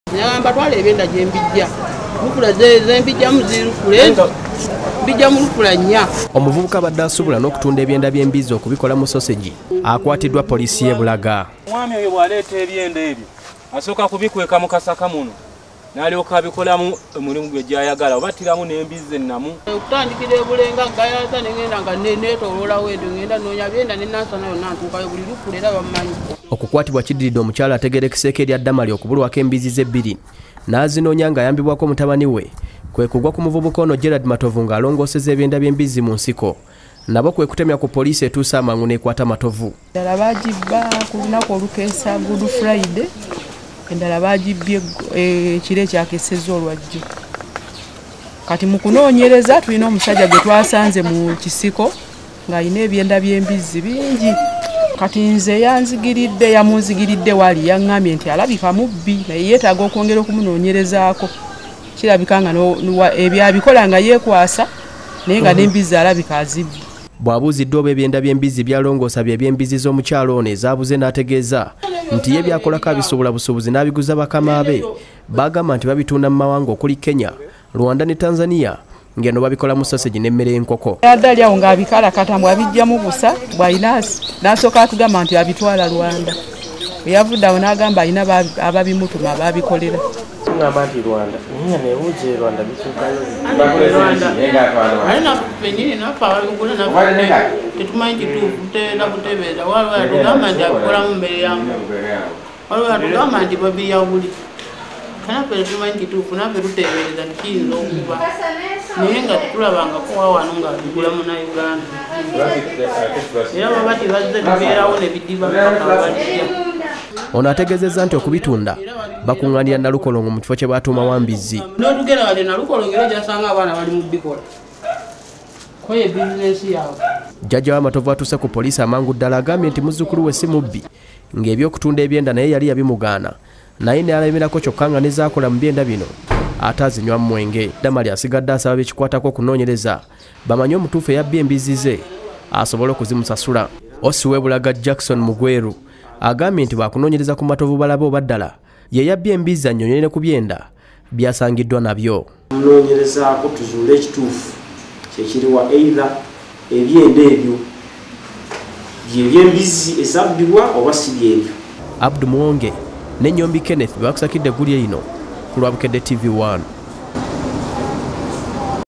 Omusajja bamukutte n'ebyenda by'embizzi
Akkirizza okubitunda ebweru w'eggwanga